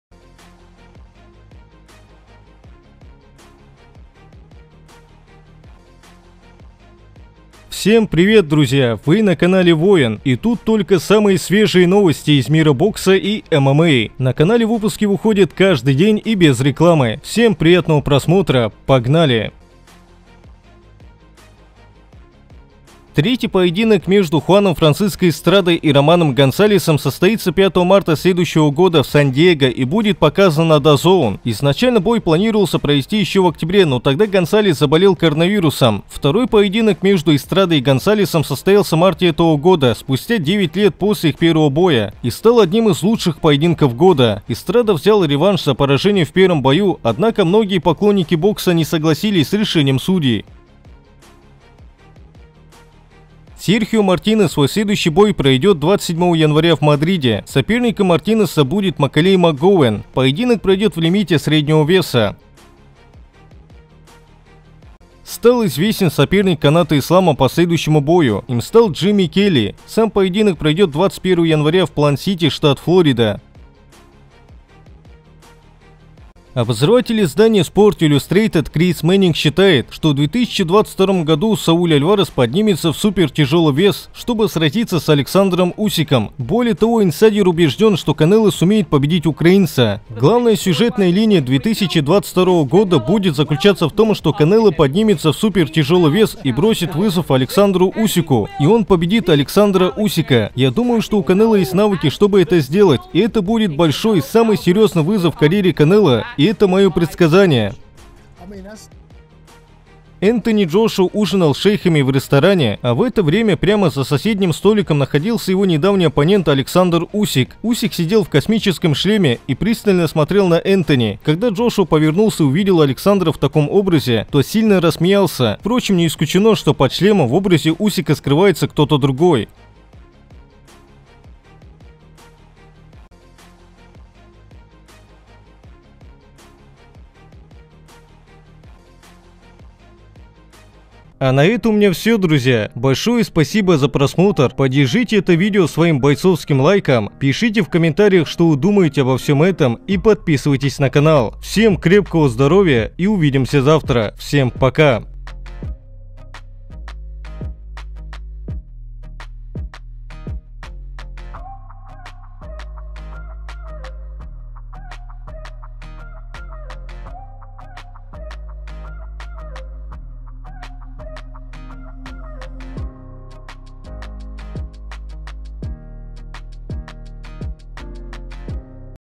ВСТРЕТИЛИСЬ!? УСИК ВЫСЛЕДИЛ ДЖОШУА НА ОТДЫХЕ! Канат Ислам Узнал Соперника
Третий поединок между Хуаном Франсиско Эстрадой и Романом Гонсалесом состоится 5 марта следующего года в Сан-Диего, и будет показан DAZN. Изначально бой планировалось провести еще в октябре, но тогда Гонсалес заболел коронавирусом.
Второй поединок между Эстрадой и Гонсалесом состоялся в марте этого года, спустя девять лет после их первого боя, и стал одним из лучших поединков года. Эстрада взял реванш за поражение в первом бою, однако многие поклонники бокса не согласились с решением судей
Серхио Мартинес свой следующий бой проведет 27 января в Мадриде Соперником Мартинеса будет Маколей Макгован. Поединок пройдет в лимите среднего веса
Стал известен соперник Каната Ислама по следующему бою, Им стал Джимми Келли Сам поединок пройдет 21 января в Плант-Сити штат Флорида,
Обозреватель издания Sports Illustrated Крис Мэнникс считает, что в 2022 году = Сауль Альварес поднимется в супертяжелый вес, чтобы сразиться с  Александром Усиком. Более того, инсайдер убежден, что Канело сумеет победить украинца.
Главная сюжетная линия 2022 года будет заключаться в том, что Канело поднимется в супертяжелый вес и бросит вызов Александру Усику. И он победит Александра Усика. Я думаю, что у Канело есть навыки, чтобы это сделать. Это будет большой, самый серьезный вызов в карьере Канело, но это мое предсказание", - заявил Крис Мэнникс на подкасте The DAZN Boxing Show.
Энтони Джошуа ужинал с шейхами в ресторане, а в это время прямо за соседним столиком находился его недавний оппонент Александр Усик,

Усик сидел в космическом шлеме и пристально смотрел на Энтони. Когда Джошуа повернулся и увидел Александра в таком образе, то сильно рассмеялся.

Впрочем, не исключено, что под шлемом в образе Усика скрывался кто-то другой...